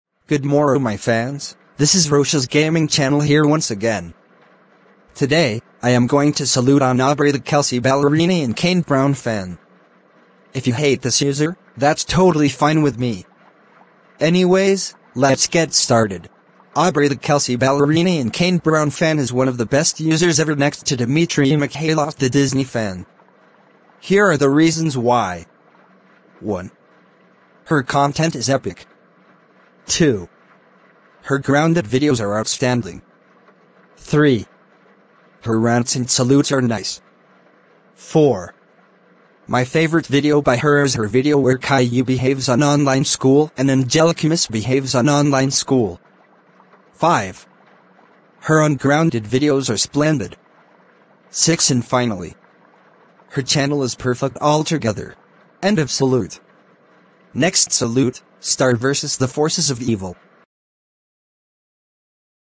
Rosh Salutes S1 E9: Aubrey the Kelsey Ballerini and Kane Brown Fan
This is my ninth salute, and I can handle your opinion if you hate Aubrey the Kelsey Ballerini and Kane Brown Fan! Note; I don't care if you bash my opinion.

Shout-out to @aubreytheautisticgirl2K2